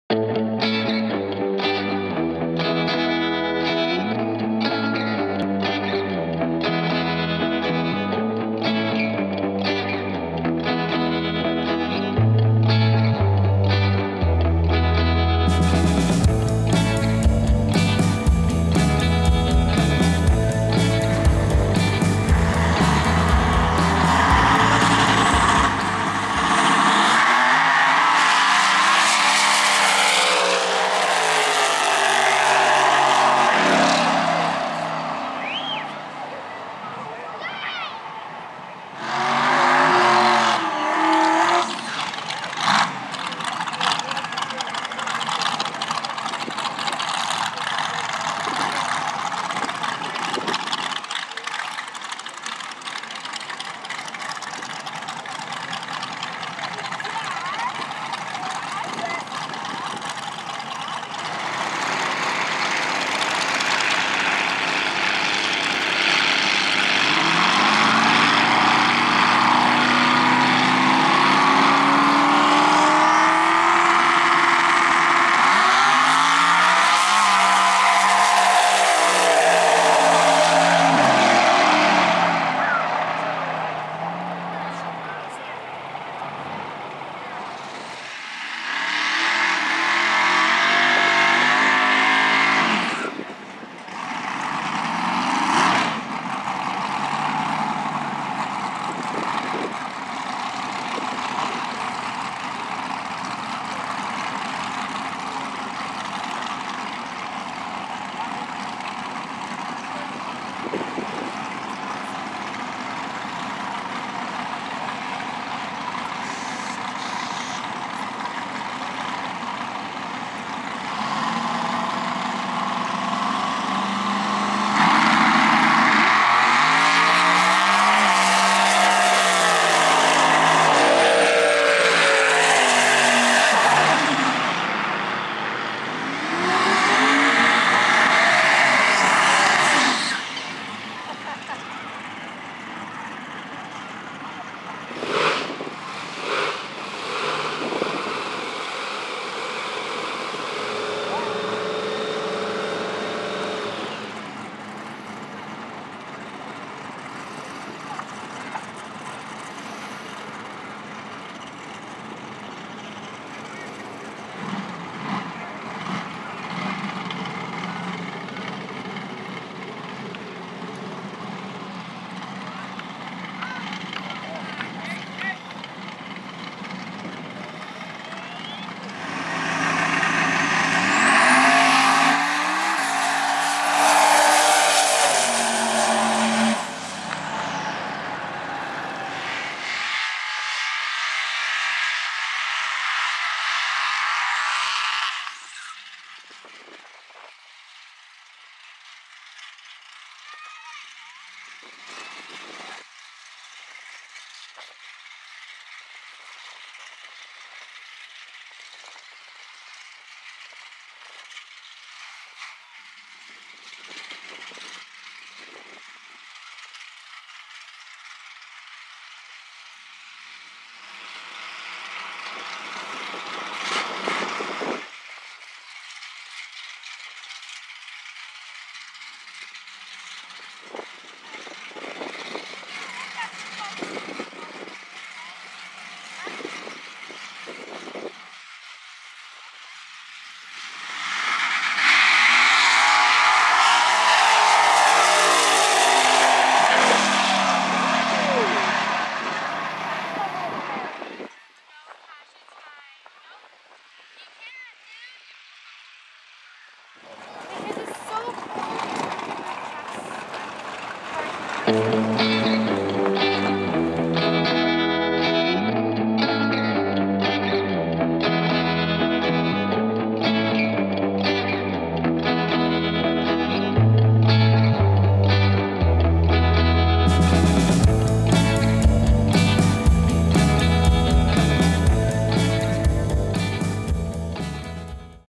WCPM x275 ROUND 3 QUALIFYING FROM CENTRAL ALBERTA RACEWAYS (7/25/2020)
Round 3 Qualifying from CARaceways in Rimbey , all I was able to shoot before the rain came, and I got out of there. (KN)

TinyURL.com/DragFiles

and like me on facebook!